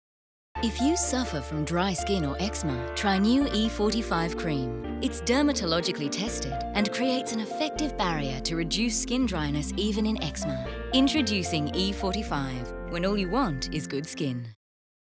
Introducing E45, When all you want is good skin
Cream E45 reduces skin dryness in conditions such as eczema.